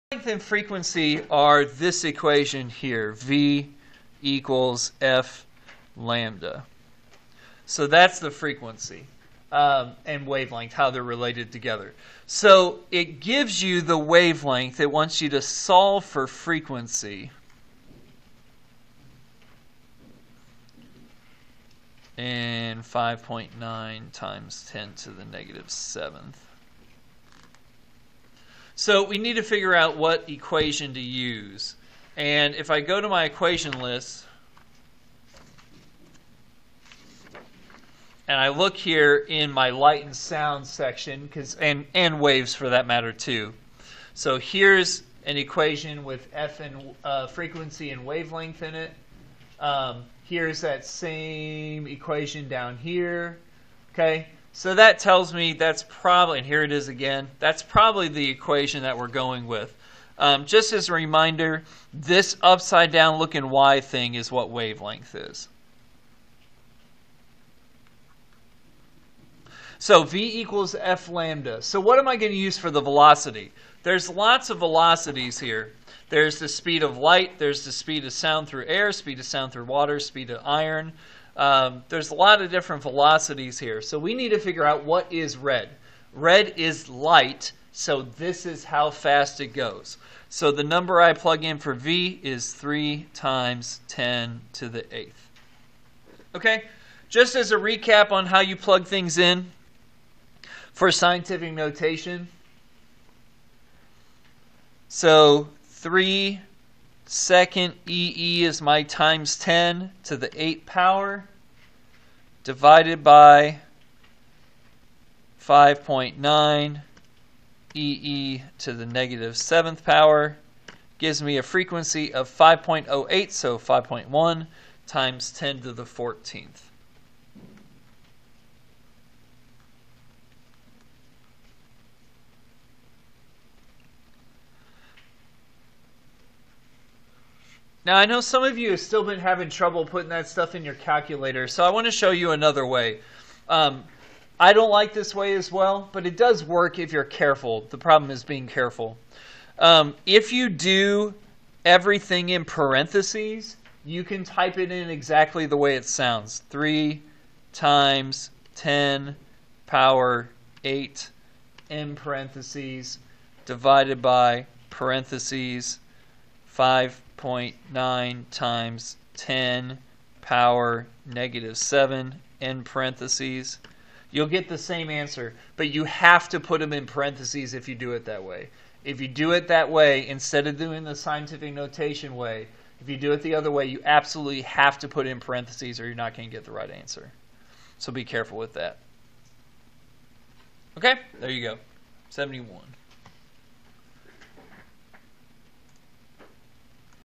Physics 1 - Final Exam Review - wave problem (#71)
Mr. B shows how to solve problems for waves